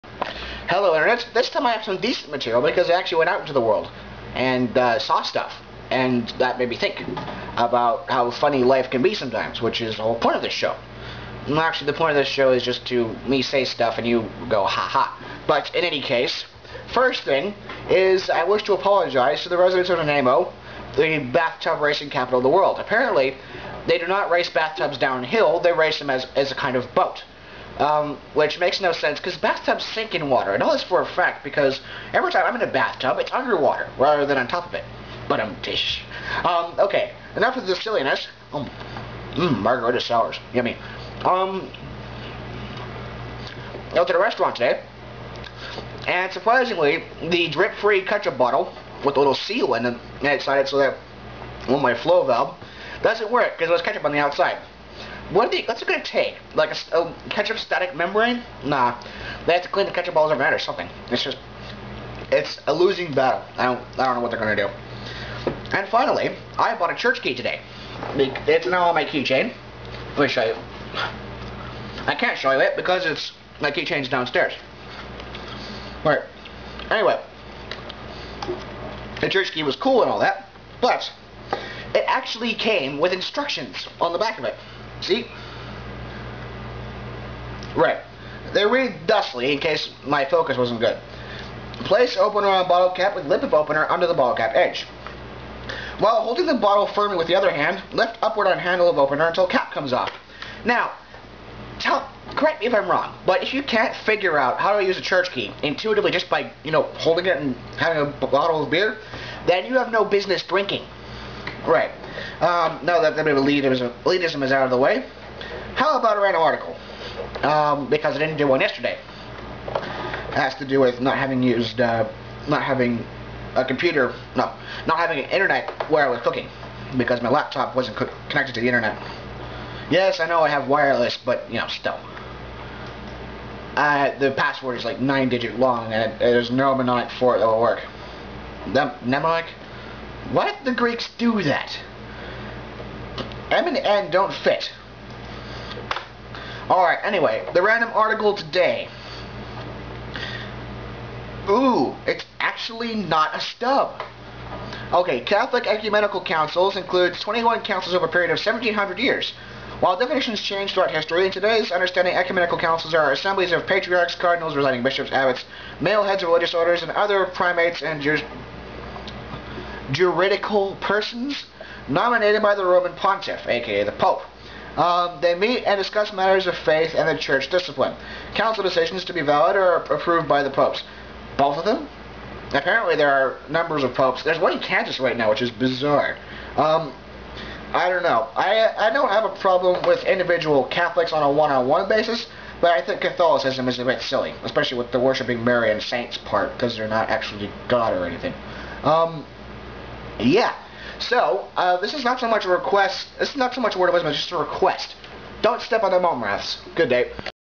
Video 59 - Bathtub Races, Ketchup, and Church Keys
The fact that I have a church key on my keyring now, is in itself, cool.  3 more days until I go home!  Also, to catholic viewers, I might make fun of other religions or Christians in a later movie, so don't yell at me.